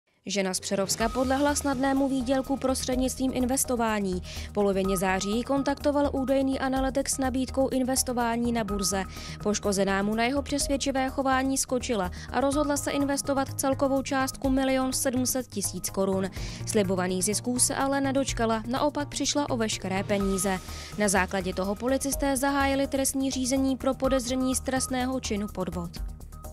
Žena z Přerovska se nechala nachytat falešným investorem
Žena z Přerovska podlehla snadnému výdělku prostřednictvím investování. V polovině září ji kontaktoval údajný analytik s nabídkou investování na burze. Poškozená mu na jeho přesvědčivé chování skočila a rozhodla se investovat celkovou částku milion sedm set tisíc korun. Slibovaných zisků se ale nedočkala, naopak přišla o veškeré peníze. Na základě toho policisté zahájili trestní řízení pro podezření z trestného činu podvod.